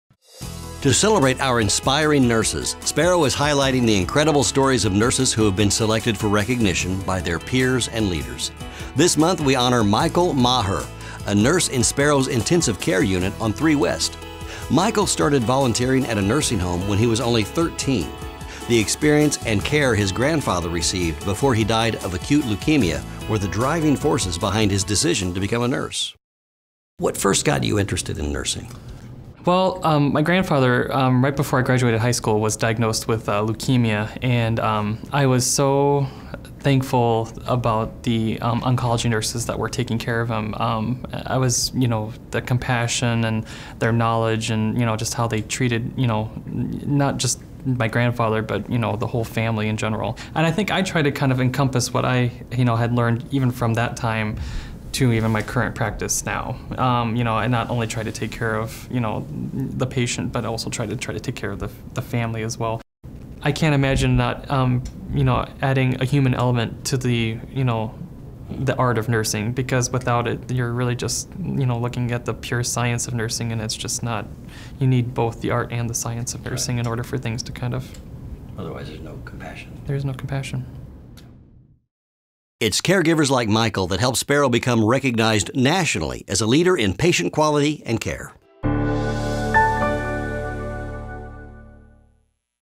RN Spotlight on Michael Maher
Our RN Caregiver Spotlight profile is on Michael Maher who was inspired by the nurses who treated his grandfather for leukemia